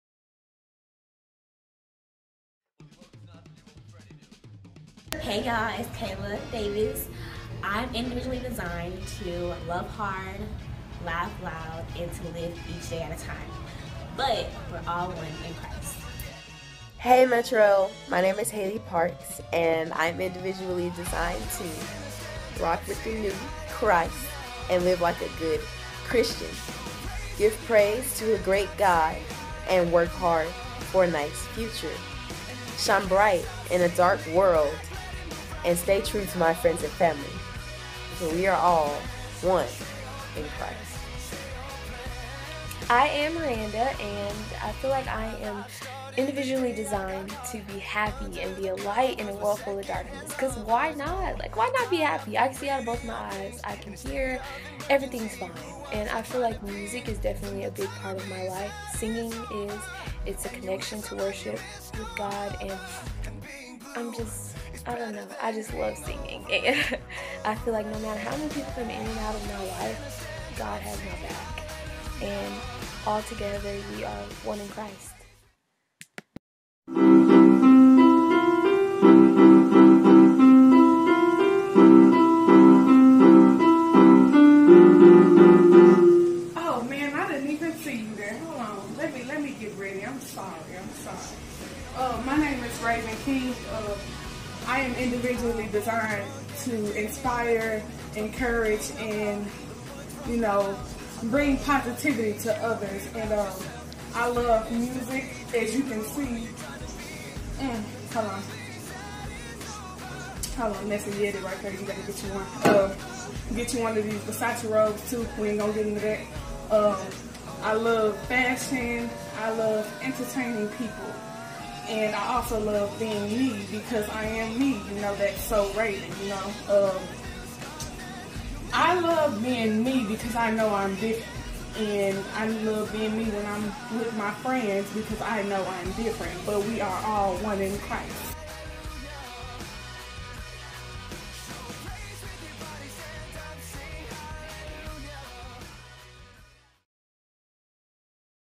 M.E.T.R.O. Christ's Church: 'Identity' Ministry
The youth ministry at M.E.T.R.O. has done it again.
Now ANYONE ages 13-19 can come enjoy being a part of the 'Identity' youth ministry!